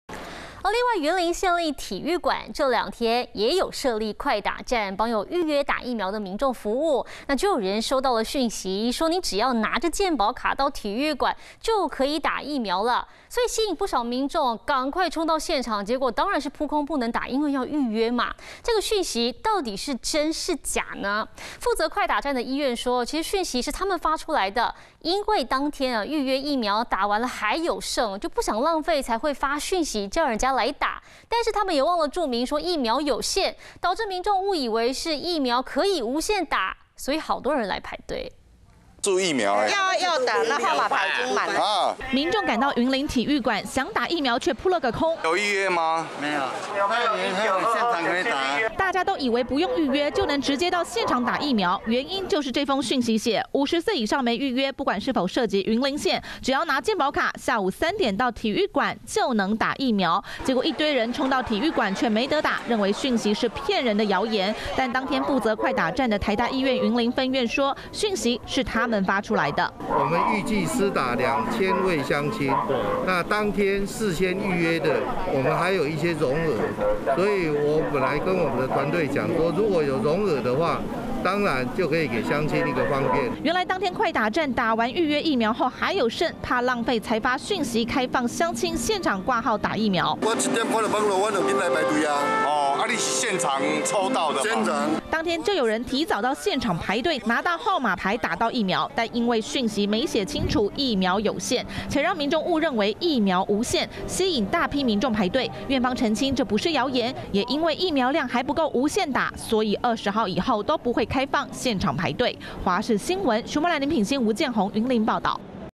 廣傳"拿健保卡就能打" 衝打潮!撲空氣炸｜華視新聞 20210720
雲林縣立體育館這兩天都有設立快打站，幫有預約打疫苗的民眾服務，就有人收到訊息說，只要帶著健保卡到體育館，就能打疫苗，吸引不少民眾衝去現場，卻撲空不能打，認為訊息是謠言，負責快打站的醫院表示，訊息是他們發出的，因為當天預約疫苗打完還有剩，不想浪費才發訊息CALL人來打，卻忘了註明「疫苗有限」，導致民眾誤以為疫苗無限，引來一堆人排隊。